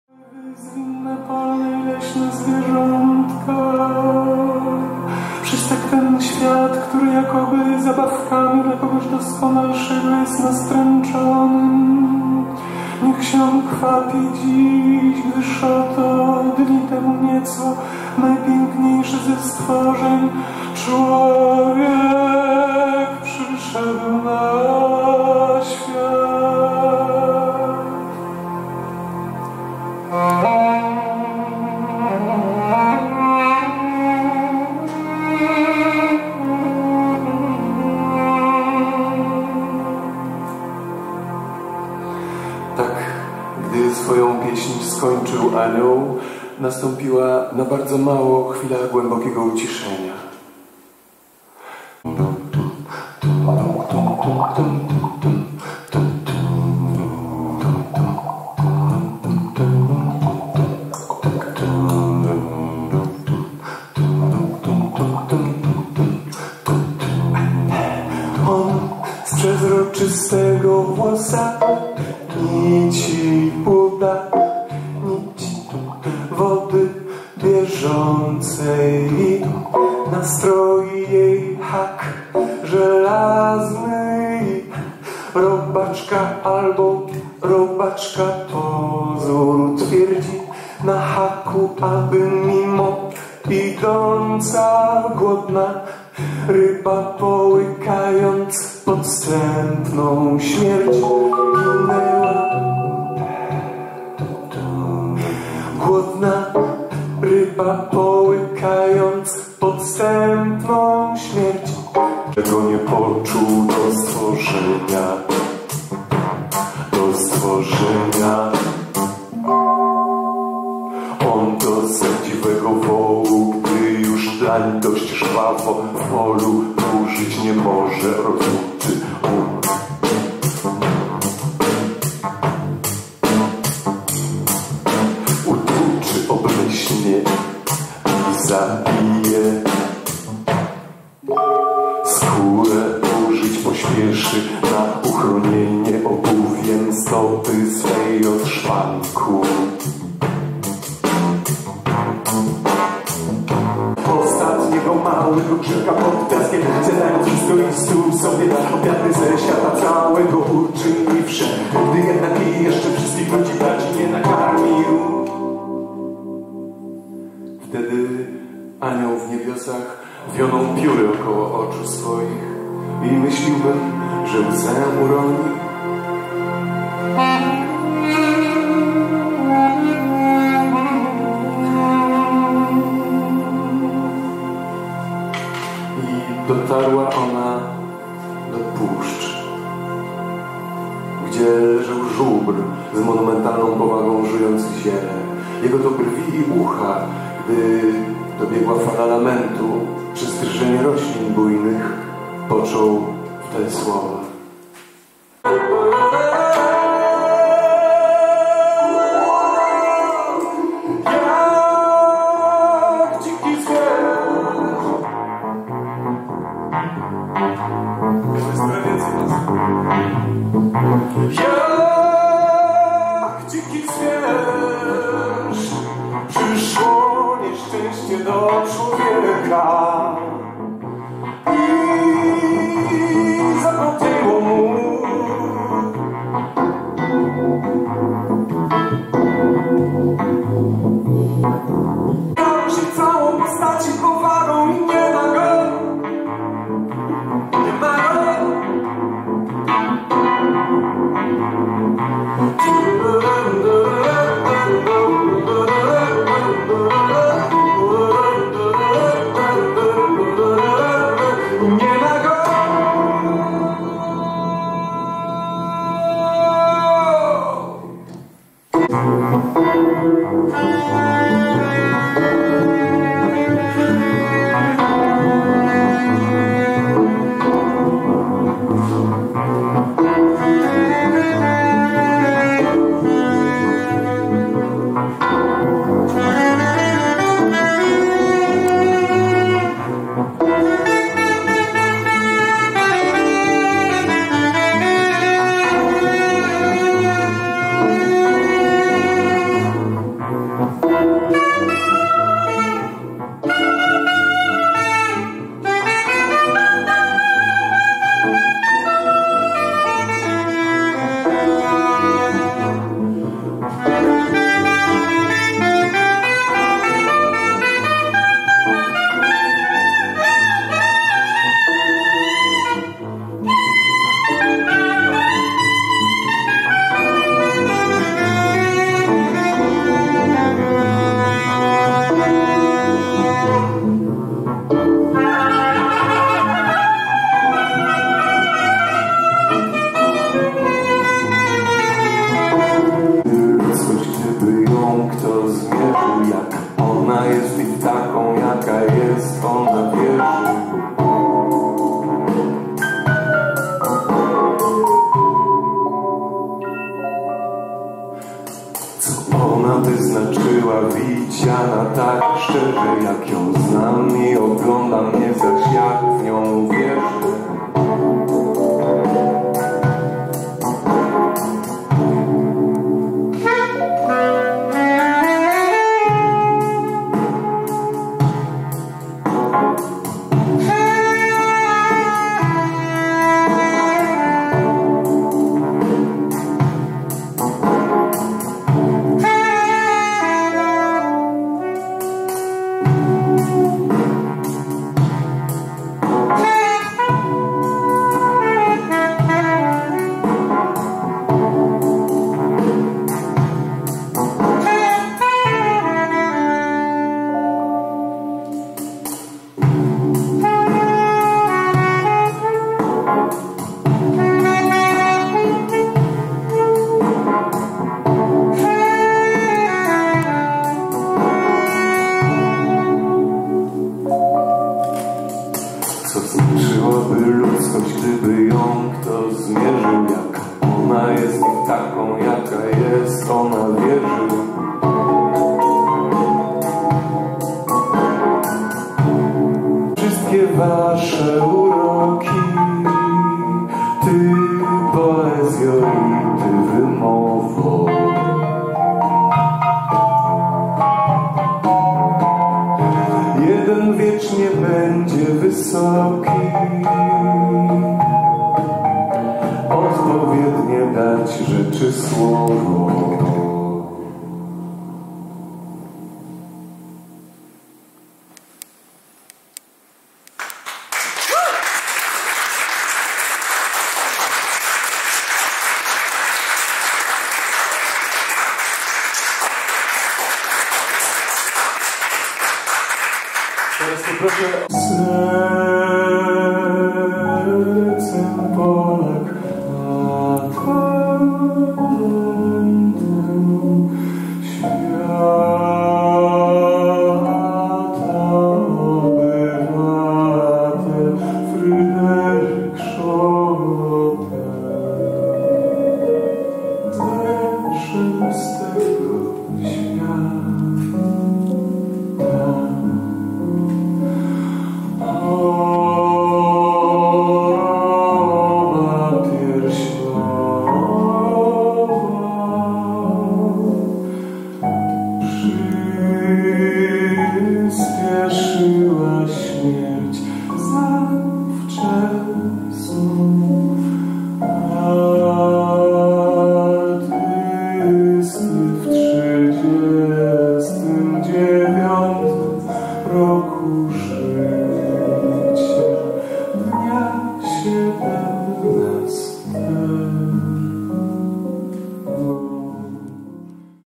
Sambor Dudziński Norwid
koncert na scenie Mazowieckiego Instytutu Kultury